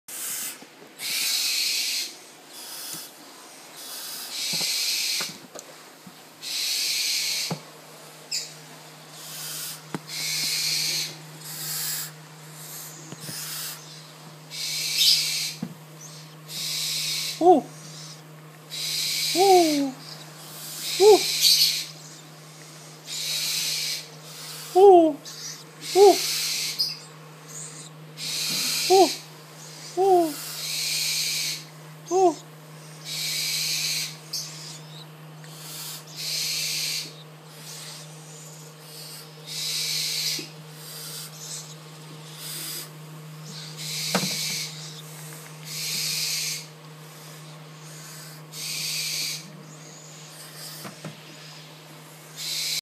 ふくろうのヒナのエサちょうだい
ふくろうと言っても、小さなコノハズクの仲間のスピックスコノハズクと言うふくろうのヒナです。
生まれた直後、いいえ、生まれる前の卵の時から大切に管理しています。

フクロウを本気で飼いたい方やご興味のある方は下記アドレスへどうぞ
By 神戸ふくろう館　猛禽倶楽部